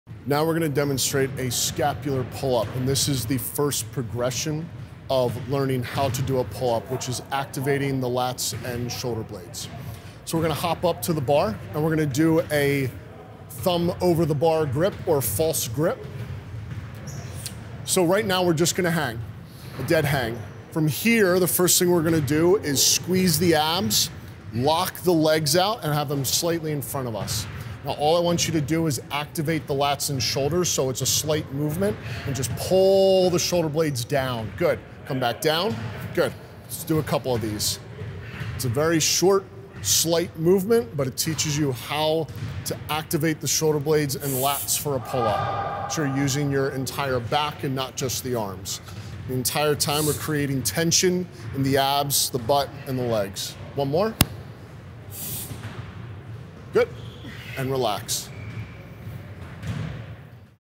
Form Demonstration: Scapular Pull-Up
Trainer Brad Feinberg walks Dan Reynolds through scapular pull-ups.